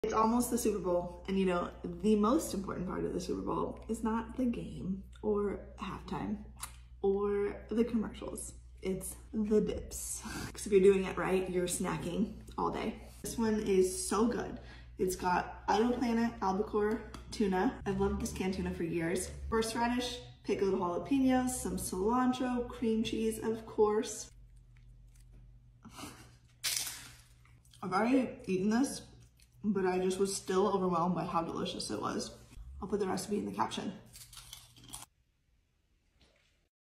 Game day appetizer inspo
Recipe by @chefalihooke

Pickled Jalapeno Tuna Dip with @wildplanetfoods

Albacore Tuna

8 ounces cream cheese, at room temperature

1/2 cup sour cream

1 tablespoon minced cilantro

1 teaspoon prepared horseradish

1/2 teaspoon kosher salt

1/4 teaspoon freshly ground black pepper

.5 cup pIckled jalapeños, chopped

Juice of 1/2 a lemon or lime

Paddle in a stand mixer until incorporated for a lighter dip or mix by hand in a large mixing bowl.